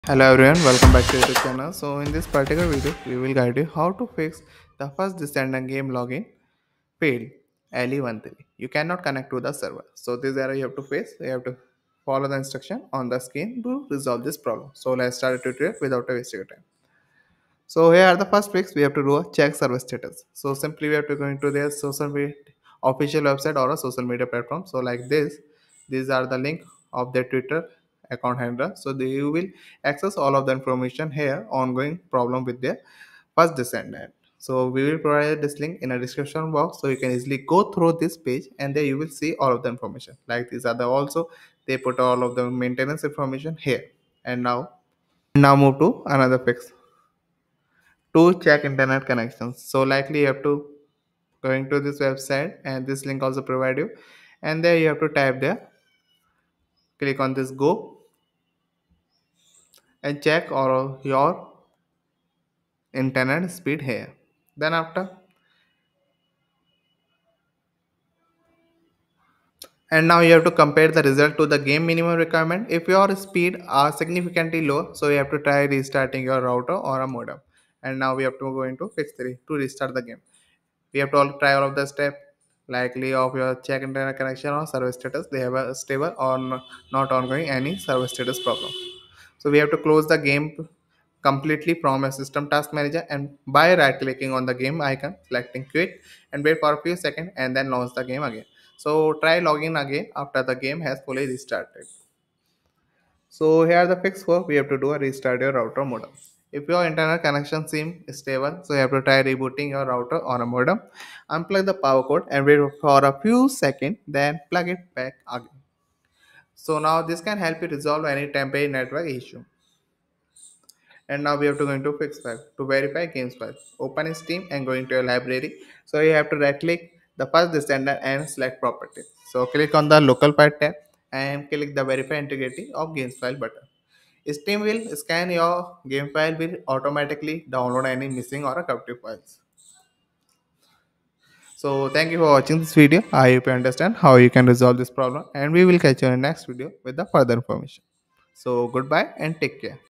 How To Fix The First Descendant Game Login Failed [Le:13] You Cannot Connect To The Server
In This Tutorial Video, Learn How To Troubleshoot And Resolve The "Login Failed [Le:13] You Cannot Connect To The Server" Issue In The Popular Game, First Descendant. Follow Step-By-Step Instructions To Fix The Problem And Get Back To Playing The Game Seamlessly.

00:19 Fix 1: Check Server Status
00:46 Fix 2: Check Internet Connection
01:18 Fix 3: Restart The Game
01:44 Fix 4: Restart Your Router/Modem
02:00 Fix 5: Verify Game Files